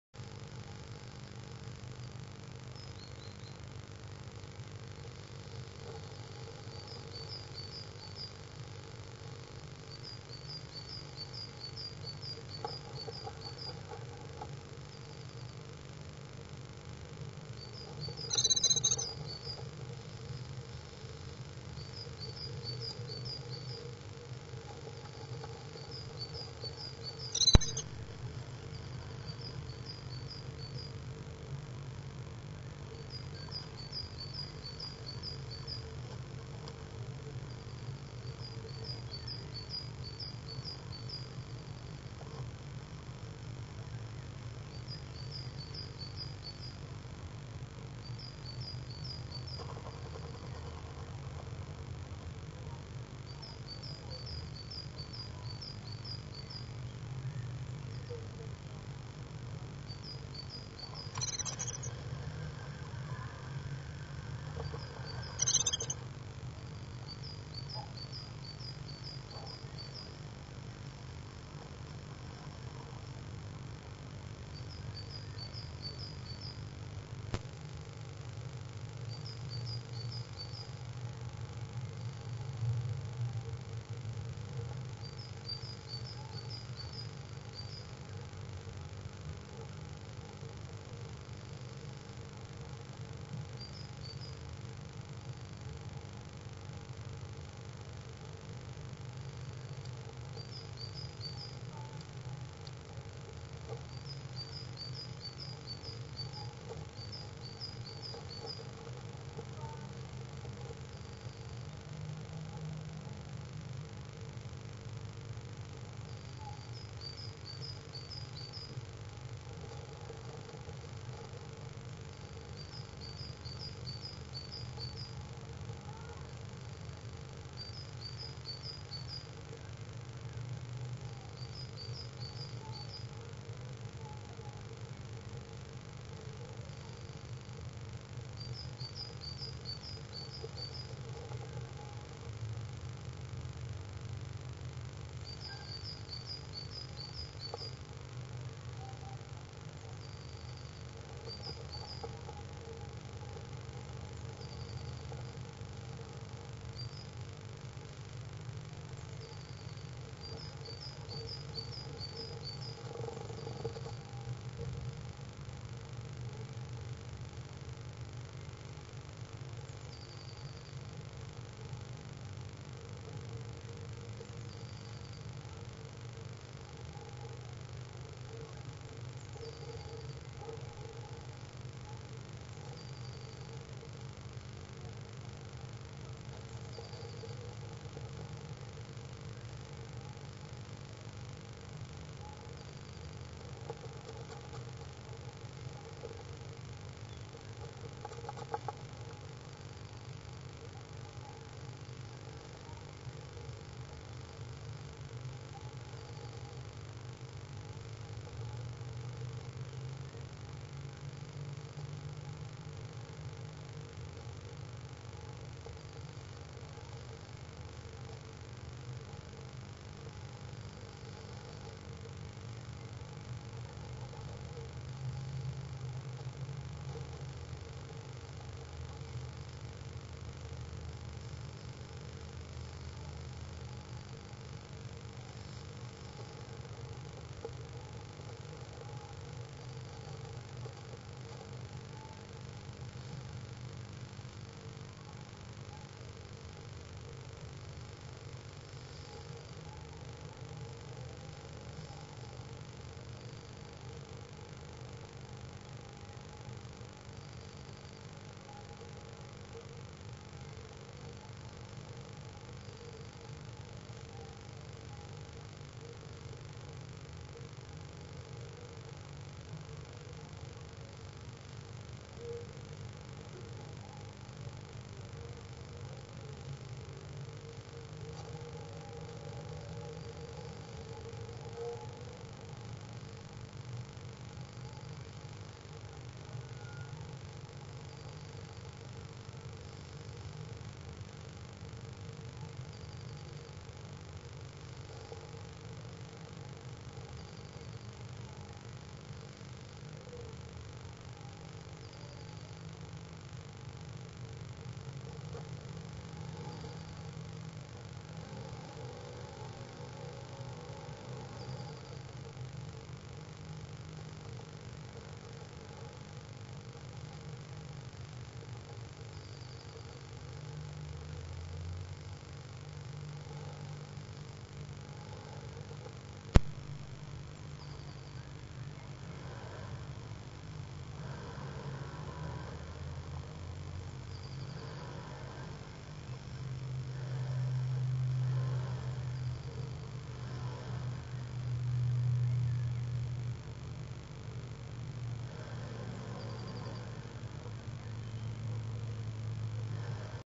Great tit preparing for the night // Koolmees maakt zich kla
Great tit preparing for the night // Koolmees maakt zich klaar voor de nacht - Captured Live on Ustream